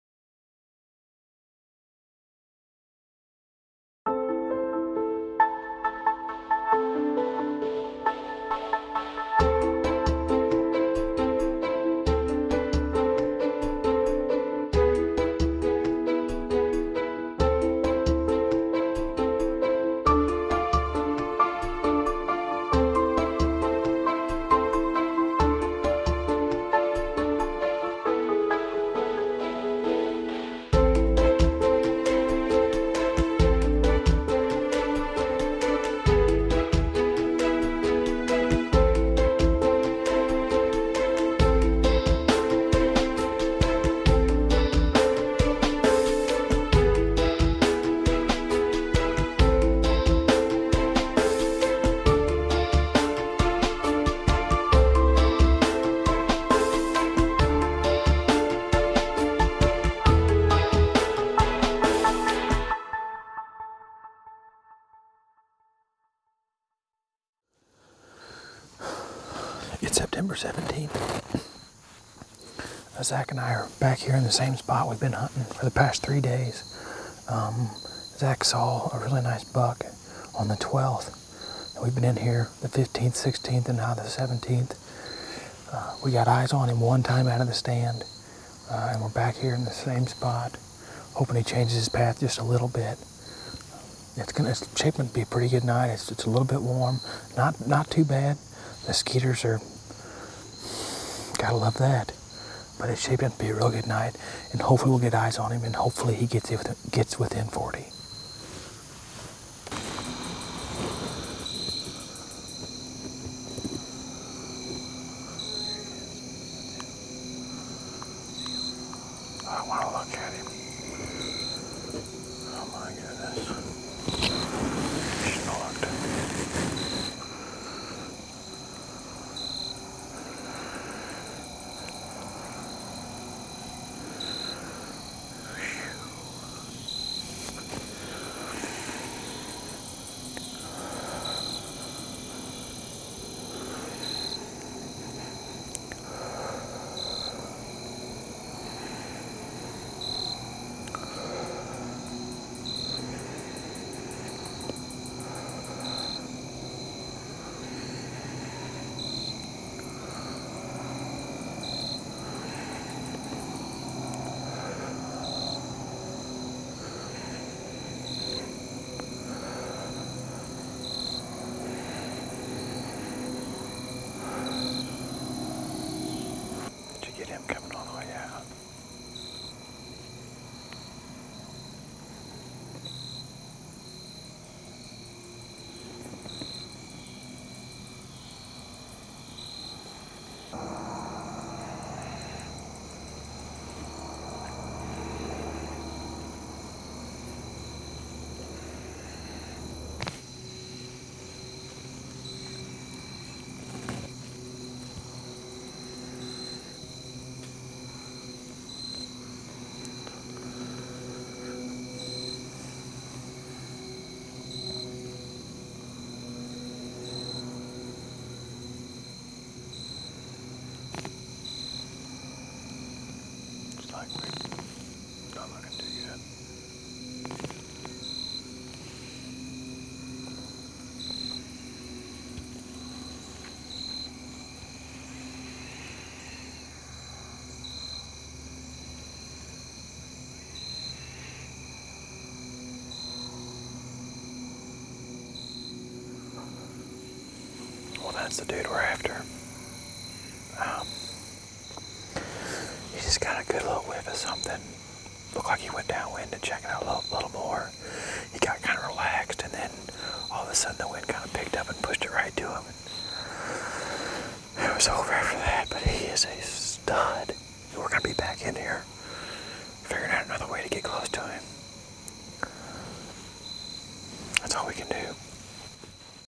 Early Season Monster Buck
Cole encounters an a true Missouri stud during the opening week of Missouri's archery season.  This buck comes out into some alfalfa with plenty of day light but just takes a wrong turn.